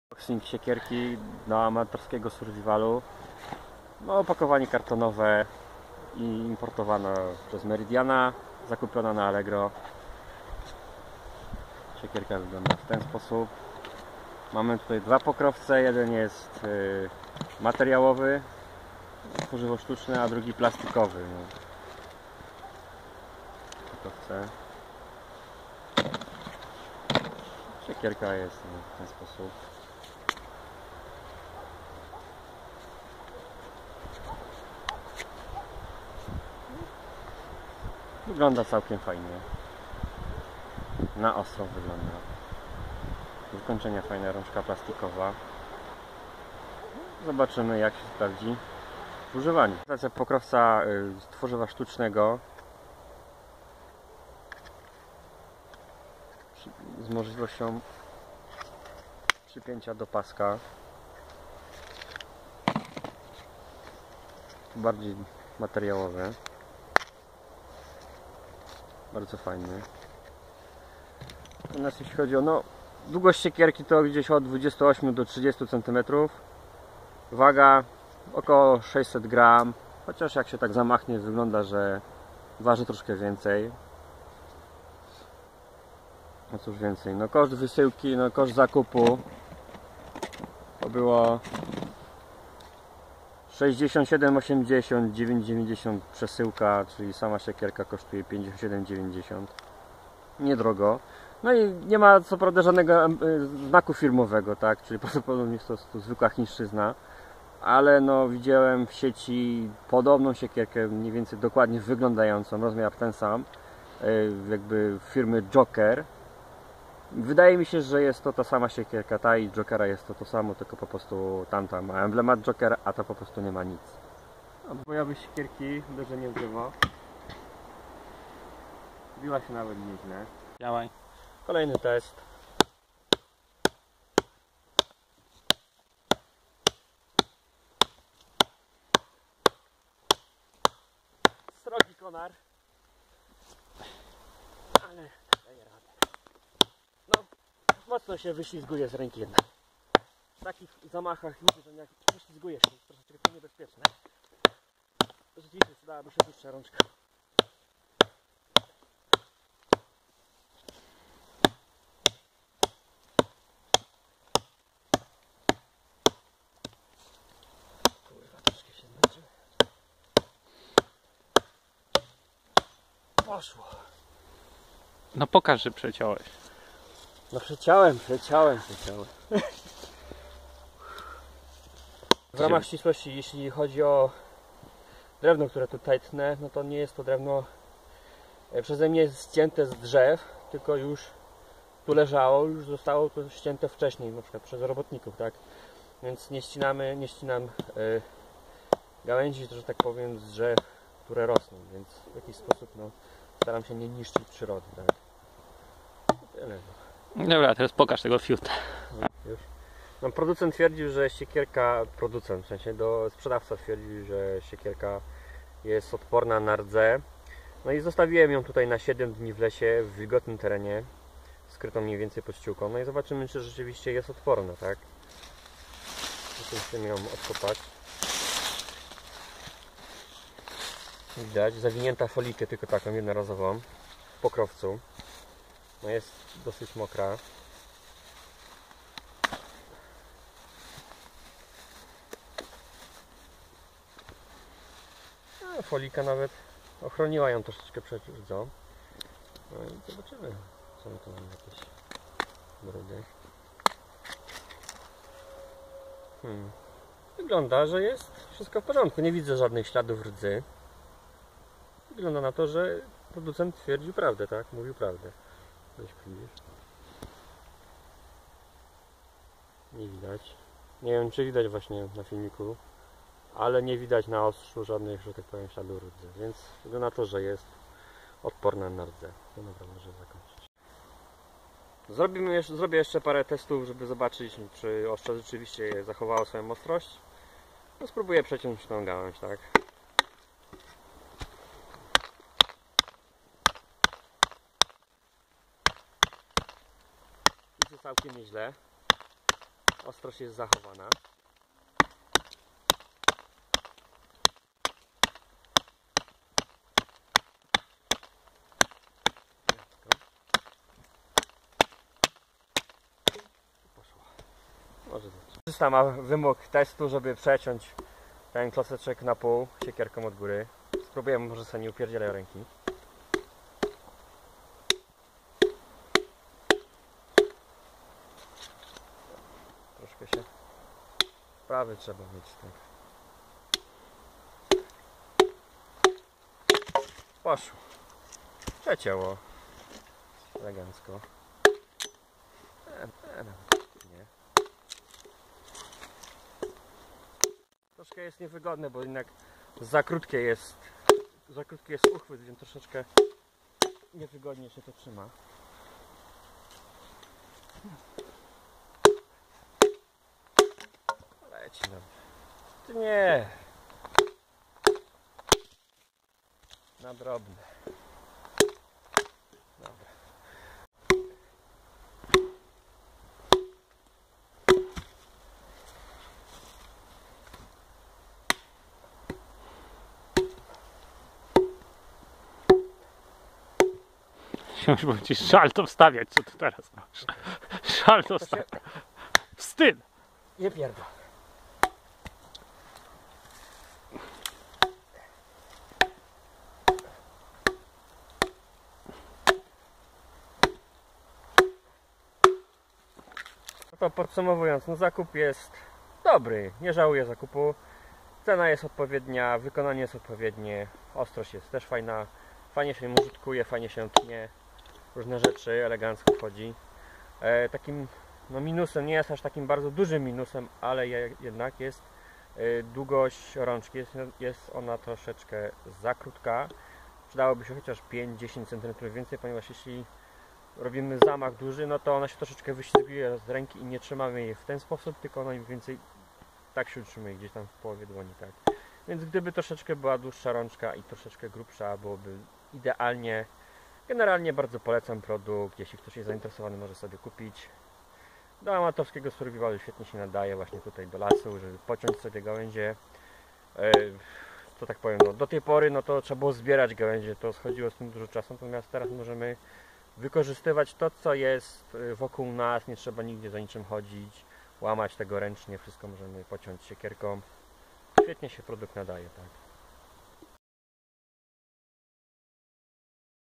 Siekiera - survival TEST
Prezentacja siekierki. Test ostrości oraz przydatności w leśnym terenie.